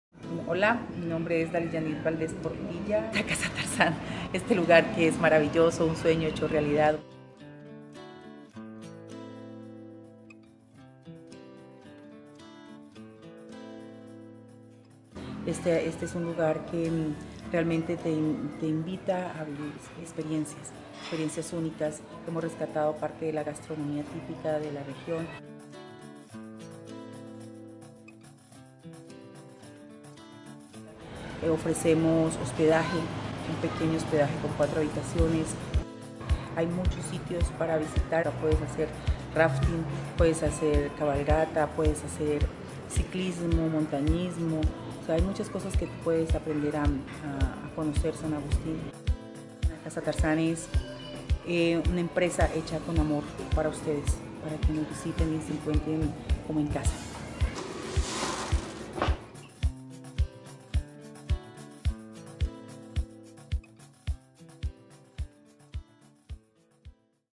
Become a lion at Casa Tarzan with Colombia Insider - San Agustin
Dally and her family offer 4 beautiful rooms in the center of San Agustin.

Each room has its own savage personality. Don't forget to dance Salsa after you ate a delicious meal at their restaurant.

Casa Tarzan is part of our strong partner network.

Are you ready to find the Tarzan in you?